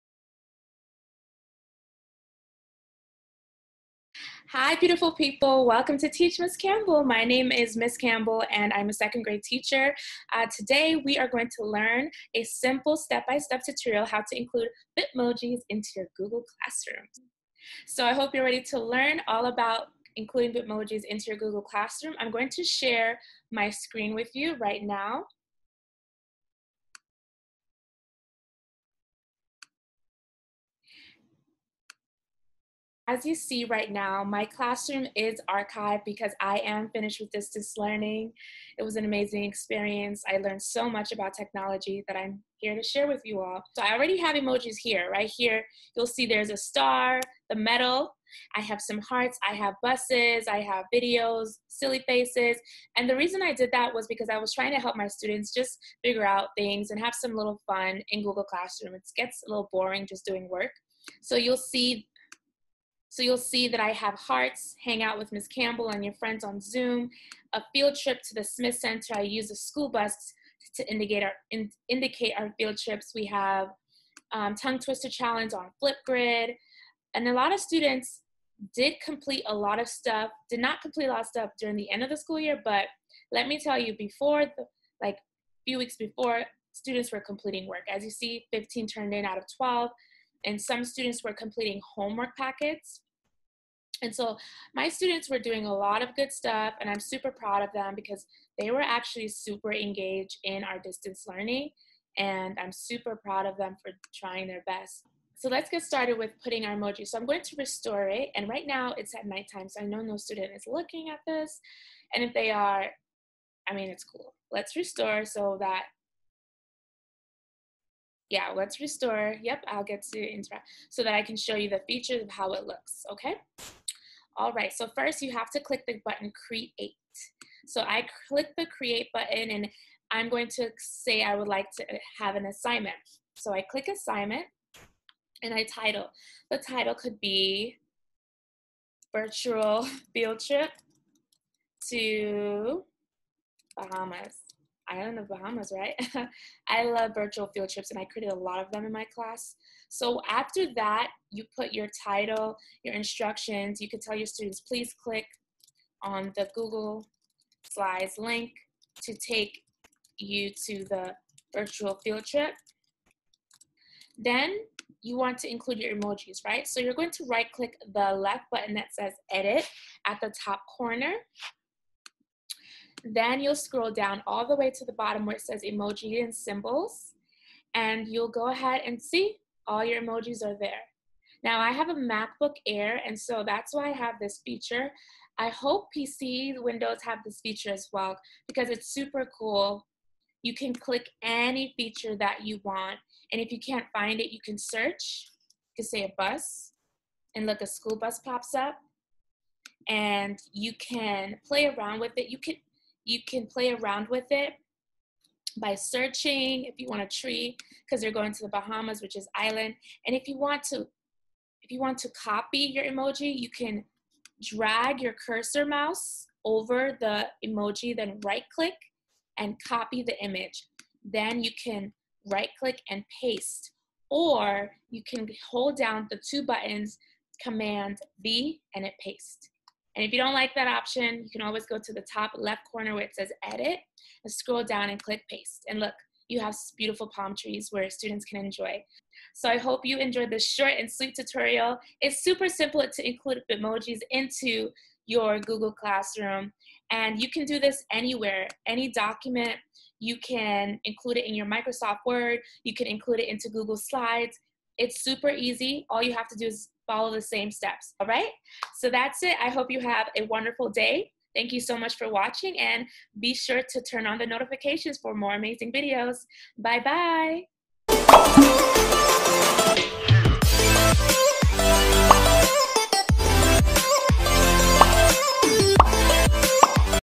How To Add EMOJIS in GOOGLE CLASSROOM - Distance Learning
Learn how to add emojis in Google classroom with Ms. Campbell! This is an EASY & DETAILED way to ADD EMOJIS 😀🤪  into your Google Classroom.👍

BEGINNING STEPS:
*0:00  Preview of my Google classroom with the Emojis
** 2:09 - Click CREATE assignment & begin to add details
*** 2:47 - Right-Click EDIT button and select Emoji & Symbol
3:23 - Different ways to search for a specific Emojii